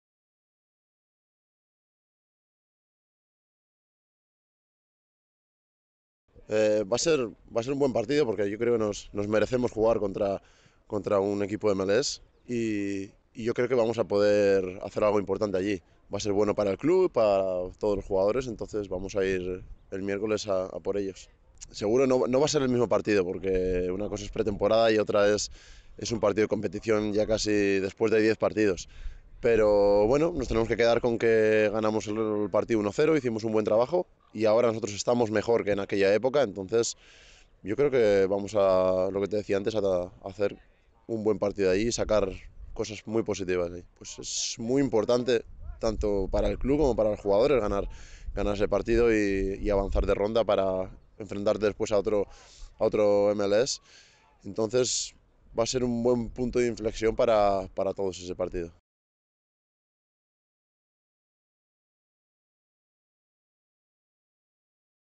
SF Deltas - (En español) Cristian Portilla discute el enfrentamiento de la Copa Abierta
El centrocampista del SF Deltas, Cristian Portilla, discute el enfrentamiento de la Copa Abierta de esta noche contra los Earthquakes de San José.